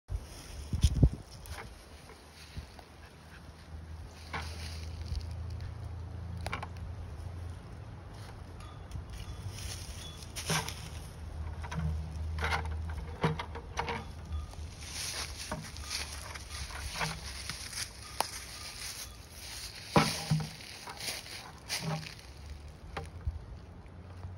Bucket Head || ViralHog
Occurred on March 16, 2022 / Columbus, Mississippi, USA

"Finkle the goat finds her party hat."